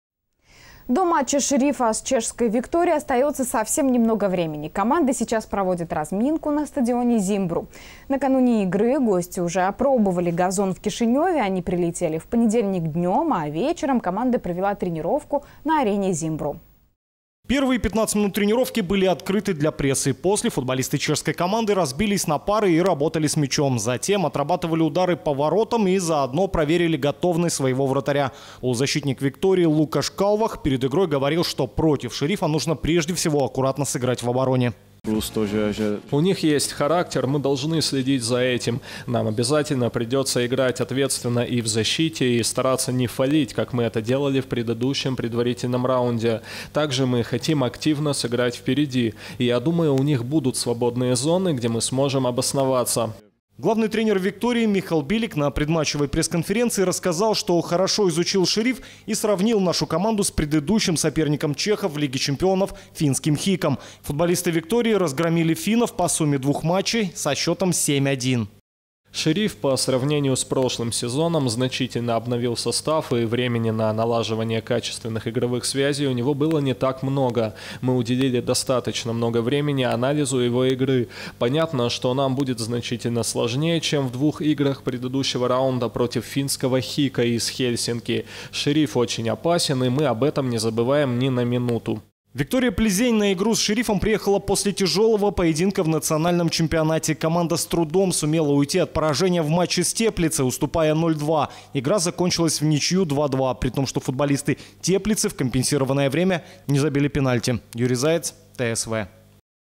«Виктория»: «Шериф» очень опасен
До матча «Шерифа» с чешской «Викторией» остаётся совсем немного времени. Команды сейчас проводят разминку на стадионе «Зимбру». Накануне игры гости уже опробовали газон в Кишиневе. Они прилетели в понедельник днем, а вечером команда провела тренировку на арене «Зимбру».